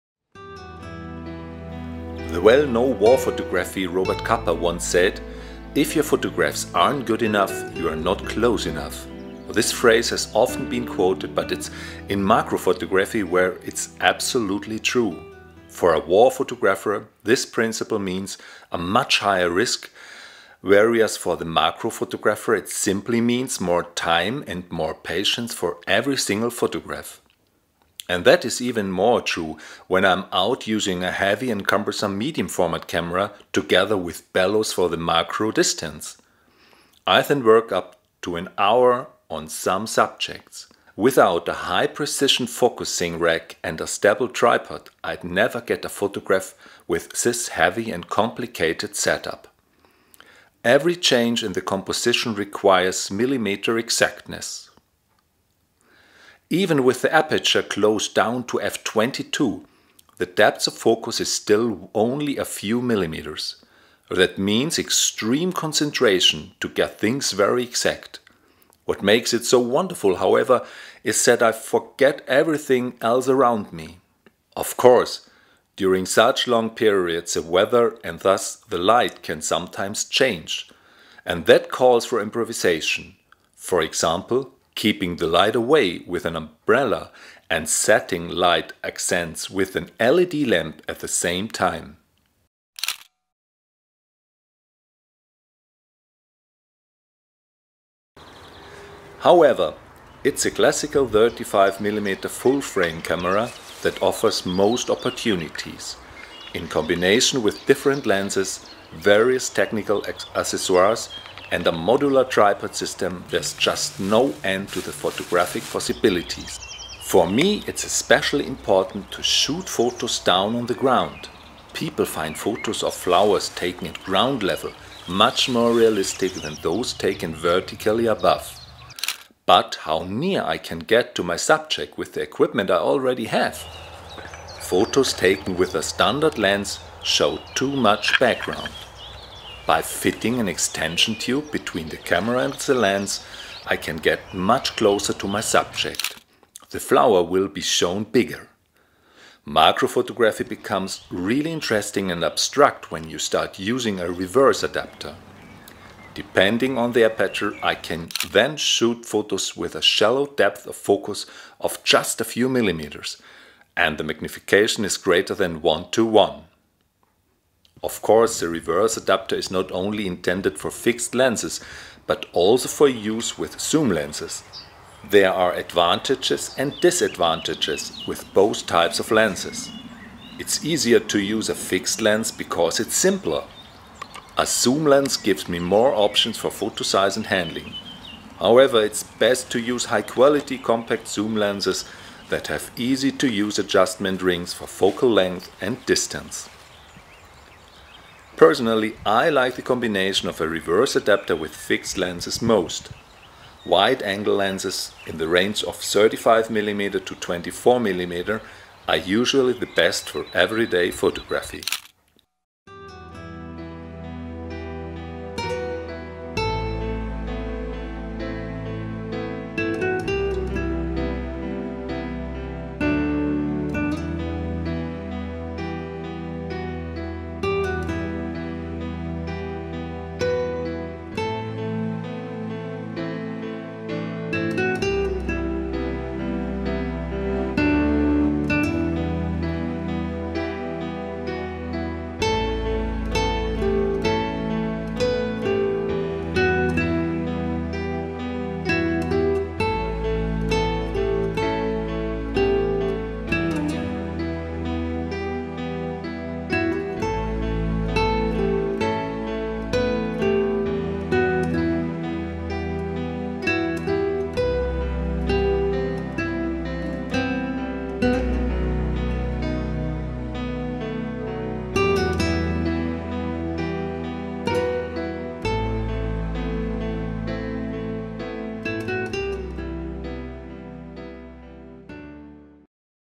NOVOFLEX Macro photography with Bernd Ritschel
Photographer Bernd Ritschel takes you on a journey into macro photography with extension tubes, bellows units, reverse rings and focusing racks. Products that only NOVOFLEX can provide in such a variety.